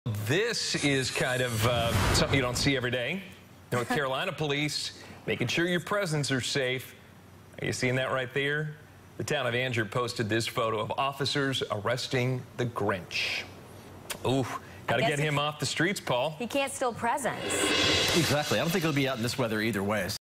Angier police arrest the Grinch
Christmas presents in Angier will be safe this year thanks to police.